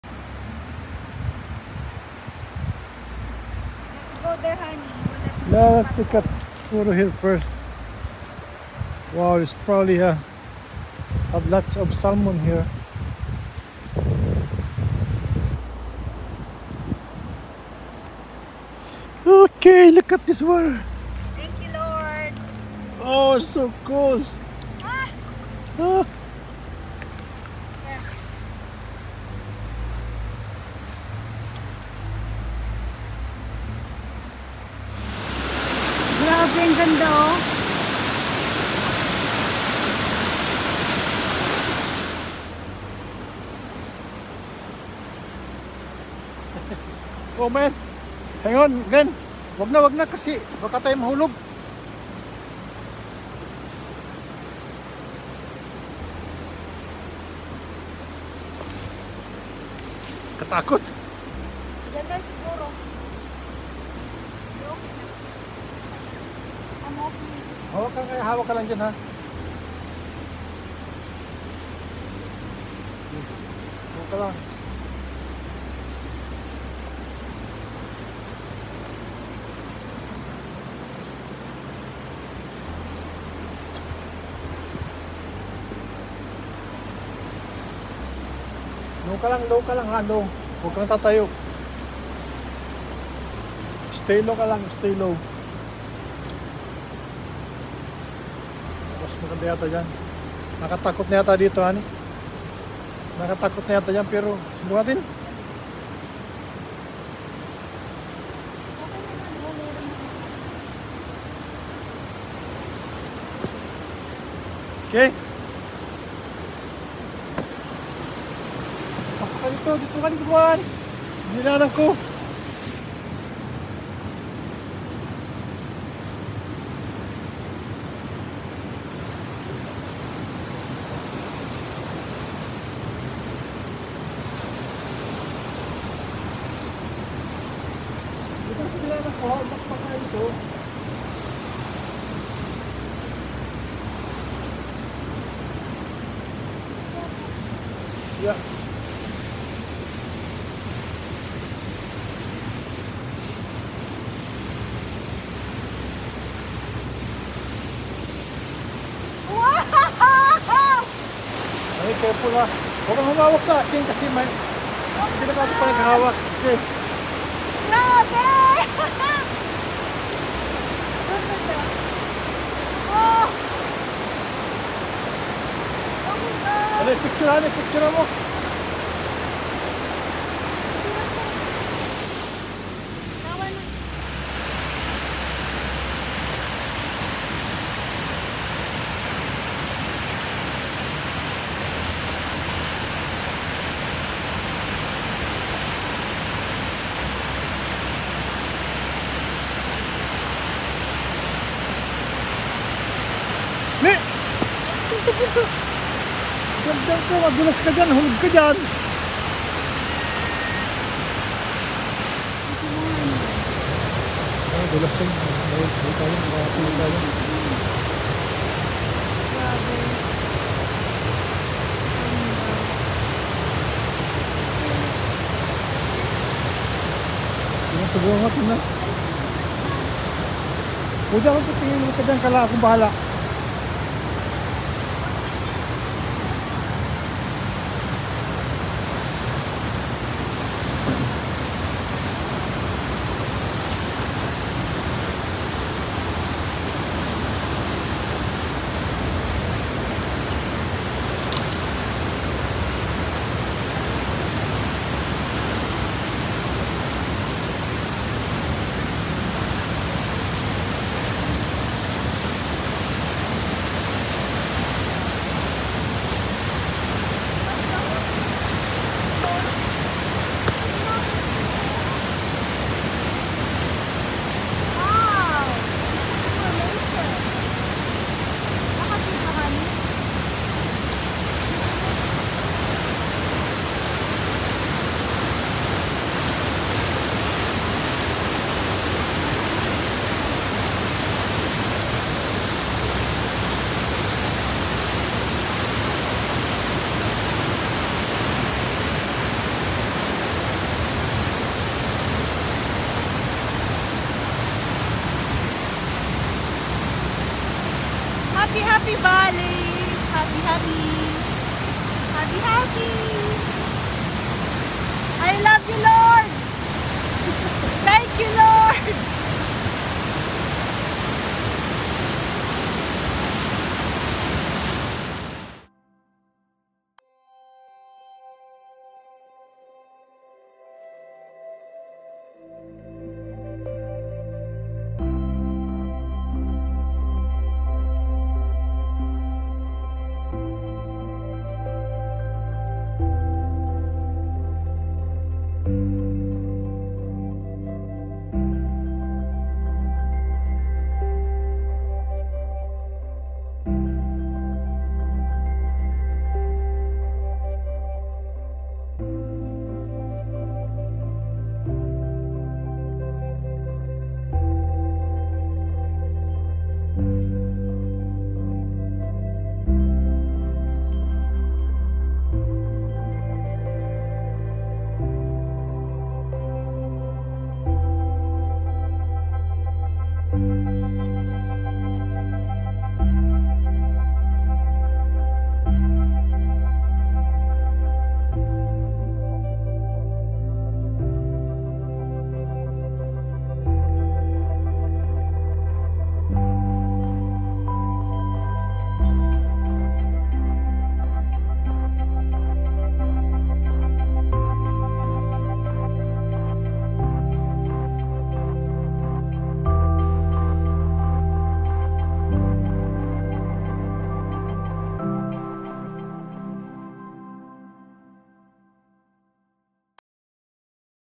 Sooke Potholes Provincial Park, Sooke, /VANCOUVER ISLAND
Sooke Potholes Provincial Park is a popular destination along the Sooke River with a unique rock formation that was formed from the last  ice- age millions of years ago.
It has several trails along the river, close to the shoreline and others that are high above the rock pools.